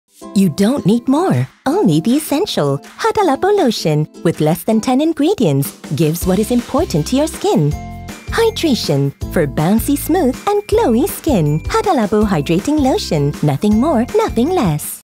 Hada Labo - Nothing More, Nothing Less
Your skin only needs the essential ingredients to give what is important to your skin. Hada Labo lotion provides hydration for a bouncy, smooth and glowing skin !